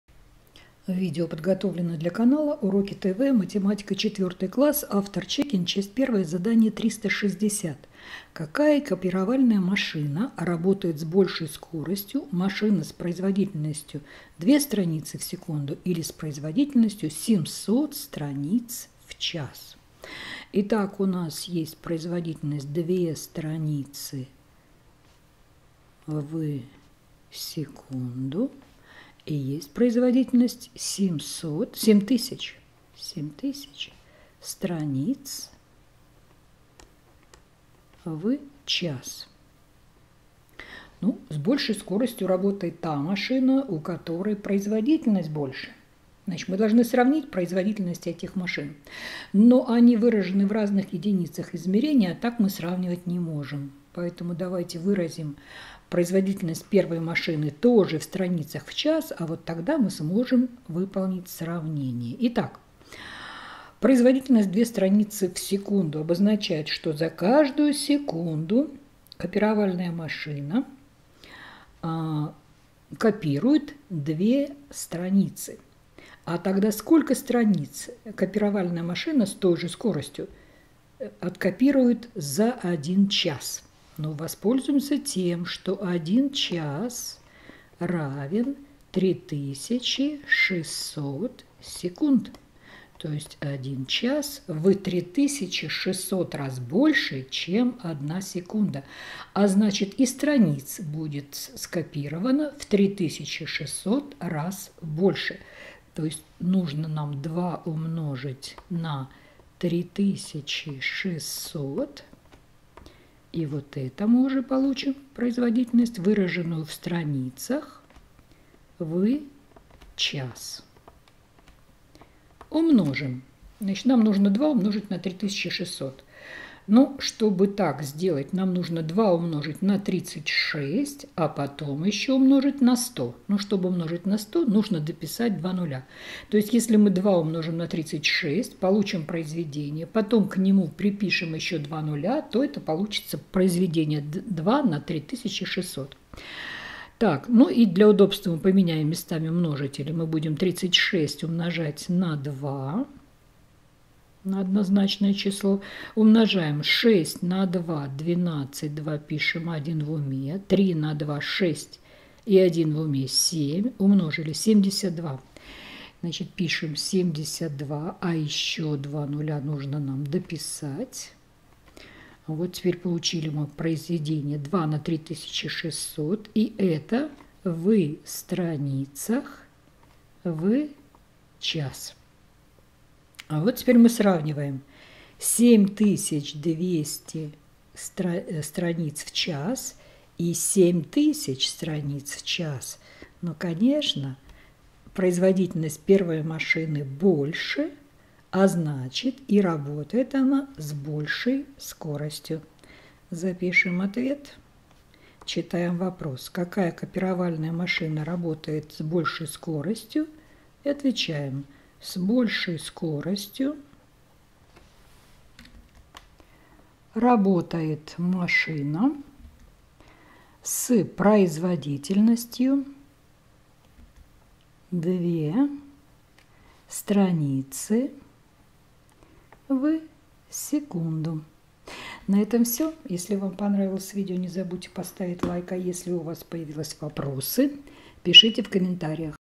Задание 360 – ГДЗ по математике 4 класс (Чекин А.Л.) Часть 1
С большей скоростью работает та копировальная машина, производительность которой больше. Для того, чтобы сравнить производительности двух копировальных машин, описанных в условии задания 360, необходимо выразить их производительности в одинаковых единицах измерения. Смотрите, как это сделано в нашем видео.